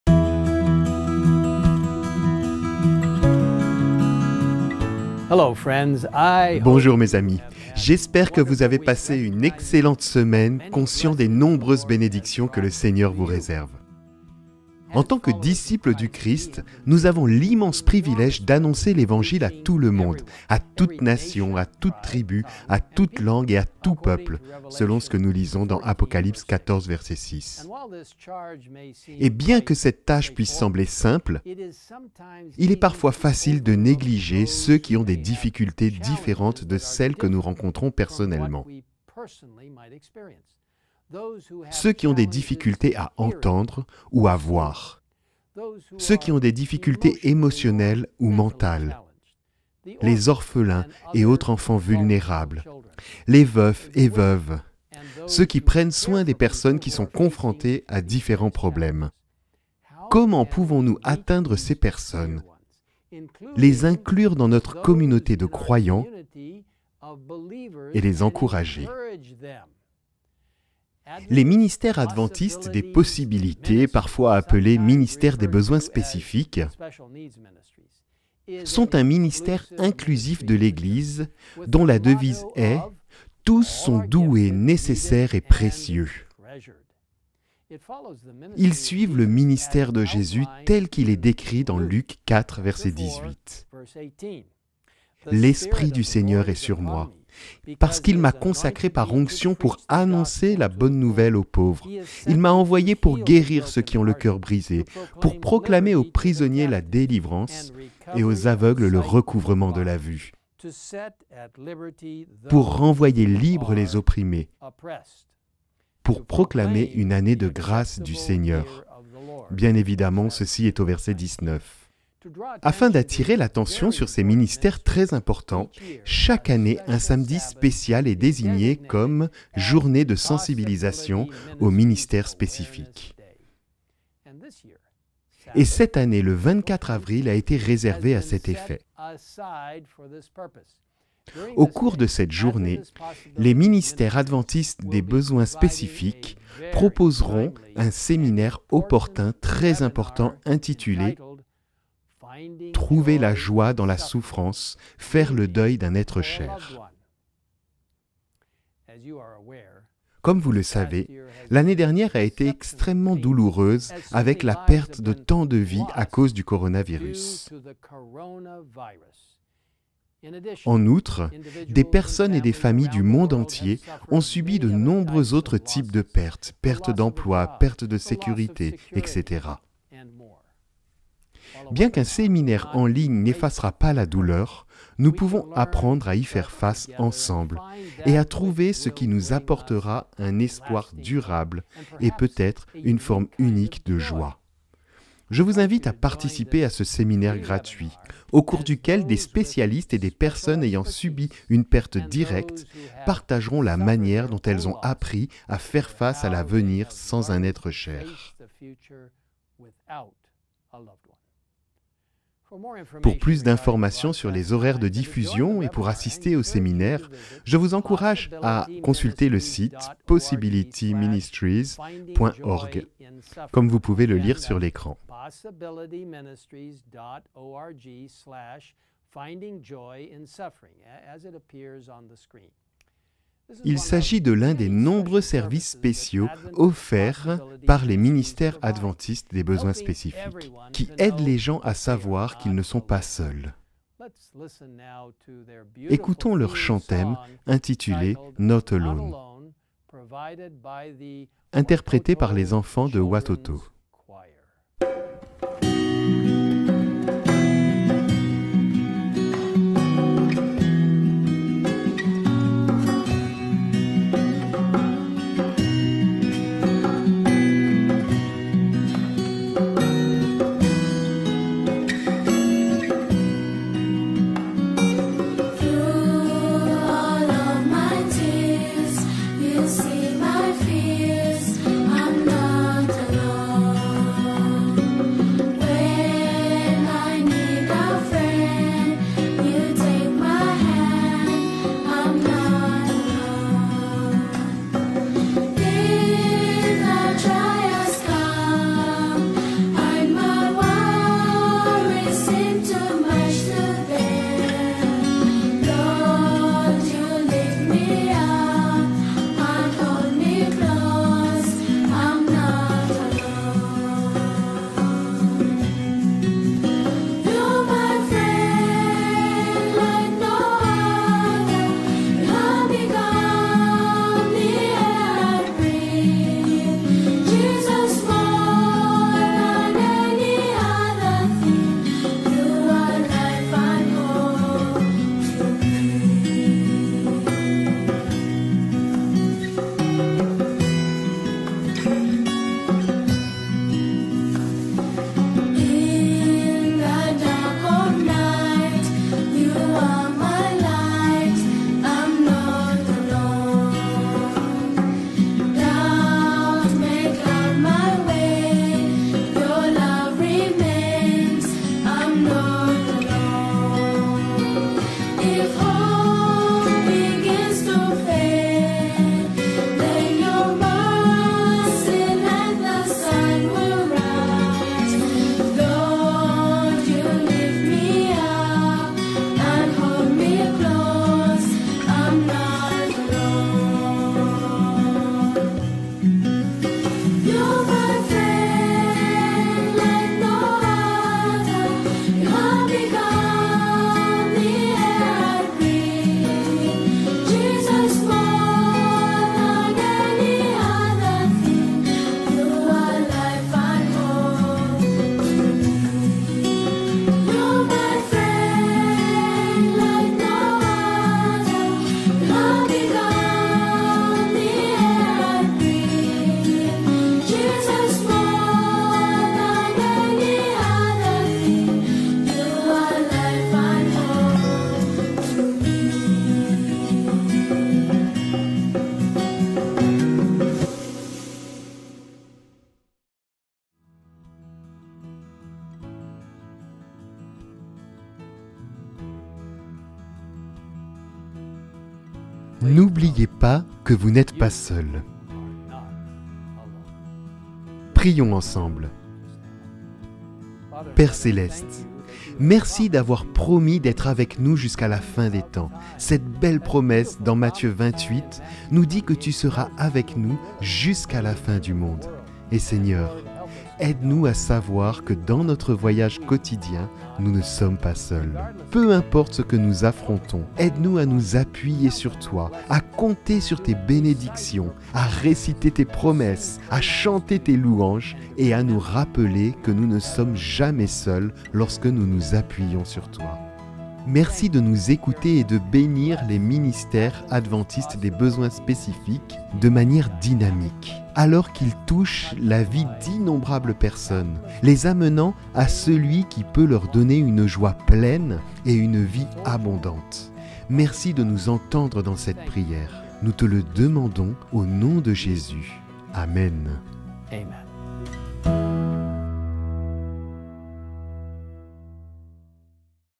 Journée du Ministère des Possibles - Message du pasteur Ted Wilson
Devenez un relayeurs d'Espoir. Recevez nos contenus en priorité

En tant que disciples du Christ, nous avons l’immense privilège d’annoncer l’Évangile à tout le monde, « à toute nation, à toute tribu, à toute langue et à tout peuple », selon ce que nous lisons dans Apocalypse 14.6. Et bien que cette tâche puisse sembler simple, il est parfois facile de négliger ceux qui ont des difficultés différentes de celles que nous rencontrons personnellement : ceux qui ont des difficultés à entendre ou à voir, ceux qui ont des difficultés émotionnelles ou mentales, les orphelins et autres enfants vulnérables, les veufs et veuves, ceux qui prennent soin des personnes qui sont confrontées à ces différents problèmes. Comment pouvons-nous atteindre ces personnes, les inclure dans notre communauté de croyants et les encourager ?

Les Ministères Adventistes des Possibilités, parfois appelés « Ministères des besoins spéciaux », sont un ministère inclusif de l’église dont la devise est : « Tous sont doués, nécessaires et précieux ! ». Afin d’attirer l’attention sur ces ministères très importants, chaque année, un samedi spécial est désigné comme « Journée de sensibilisation aux Ministères des Possibilités ». Et cette année, le sabbat 24 avril a été réservé à cet effet. Au cours de cette journée, les Ministères Adventistes des Possibilités proposeront un séminaire opportun et très important intitulé « Trouver la joie dans la souffrance : faire le deuil d’un être cher ».

L’année dernière a été extrêmement douloureuse avec la perte de tant de vies à cause du coronavirus. En outre, des personnes et des familles du monde entier ont subi de nombreux autres types de pertes : perte d’emploi, perte de sécurité, etc.

Bien qu’un séminaire en ligne n’effacera pas la douleur, nous pouvons apprendre à y faire face ensemble et à trouver ce qui nous apportera un espoir durable et peut-être une forme unique de « joie ». Je vous invite à participer à ce séminaire gratuit, au cours duquel des spécialistes et des personnes ayant subi une perte directe partageront la manière dont elles ont appris à faire face à l’avenir sans un être cher. Pour plus d’informations sur les horaires de diffusion et pour assister au séminaire, je vous encourage à consulter le

Il s’agit de l’un des nombreux services spéciaux offerts par les Ministères Adventistes des Possibilités, qui aide les gens à savoir qu’ils ne sont pas seuls. N’oubliez pas que vous n’êtes pas seul !